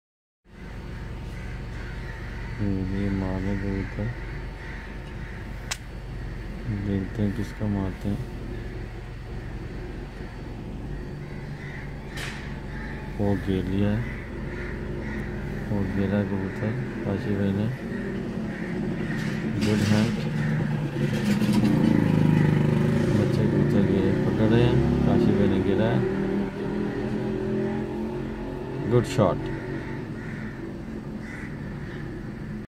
wild pigeon Hunting in pakistan
This is a hunting channel non hunter plz avoid bad comments,I am hunter all legal hunting with permit license give a wildlife of Pakistan any season ist October to 31 March